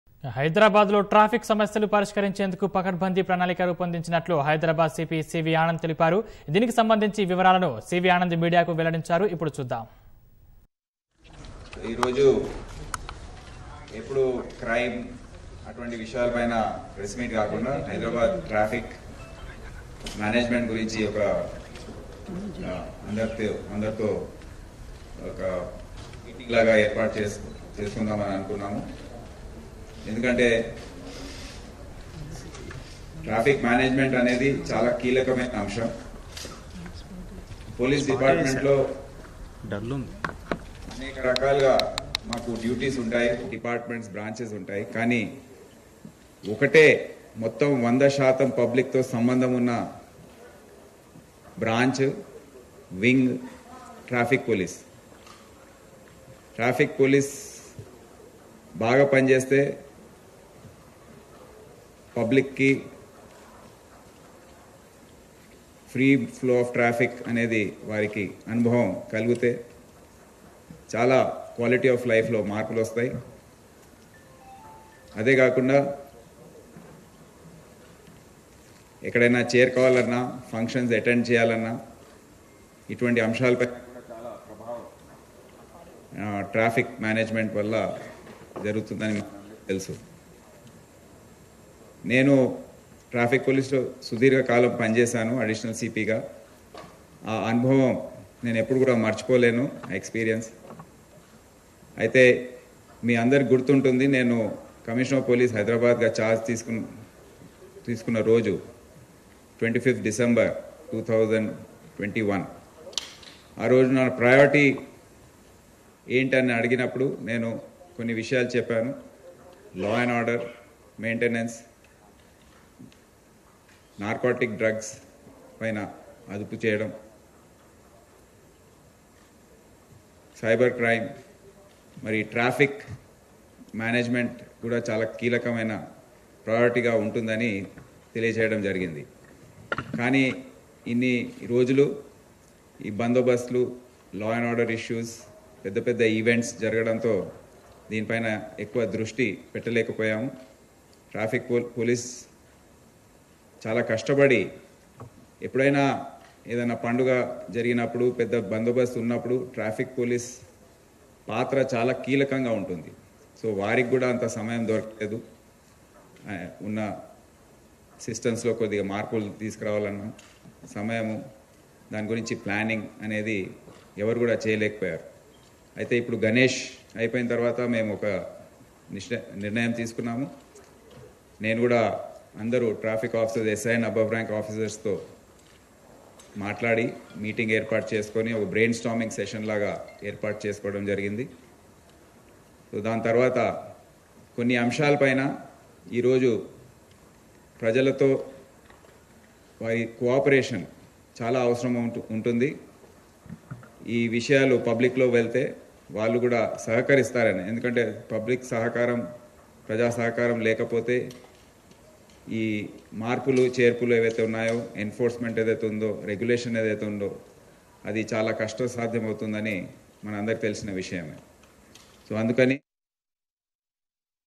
CP CV Ananad | Planned Schedule For Solving Traffic Problems | Hyderabad
హైదరాబాద్ లో ట్రాఫిక్ సమస్యలు పరిష్కరించేందుకు పకడ్బందీ ప్రణాళిక రూపొందించినట్లు హైదరాబాద్ సీపీ సీవీ ఆనంద్ తెలిపారు.